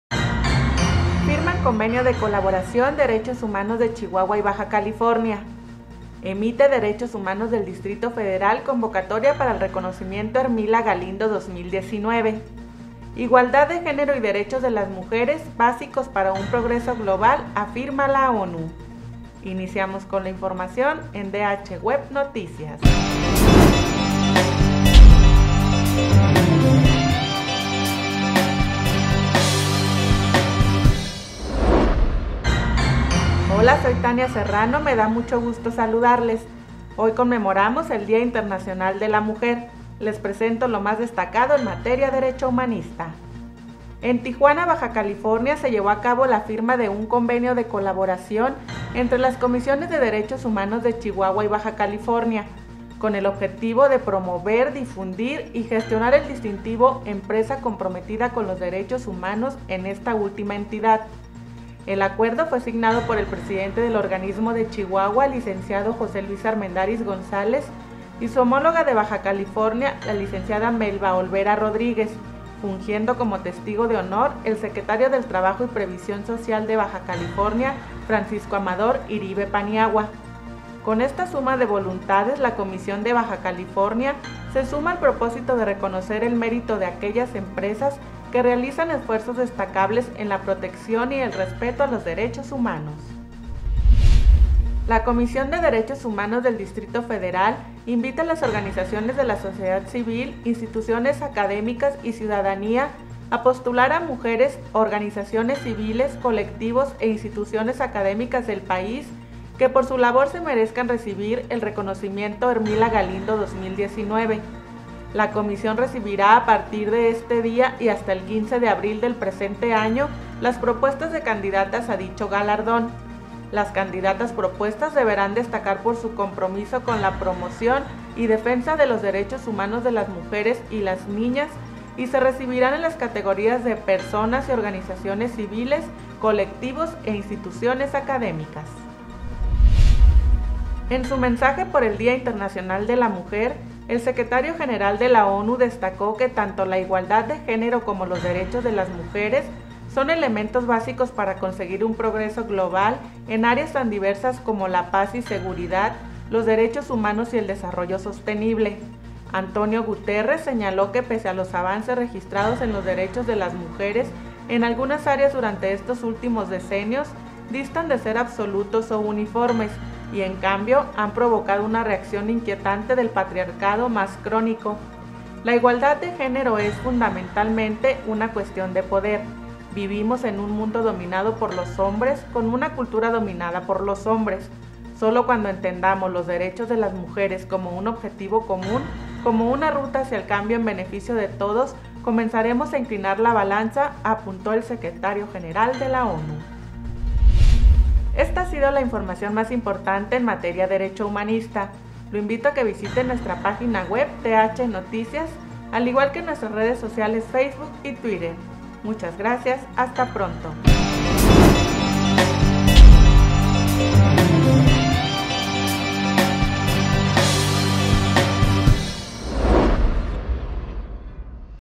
Firman convenio de colaboración Derechos Humanos de Chihuahua y Baja California
Emite Derechos humanos del Distrito Federal   convocatoria para el reconocimiento "Hermila Galindo" 2019

Igualdad de género y Derechos de las mujeres, básicos para un progreso global, afirma la ONU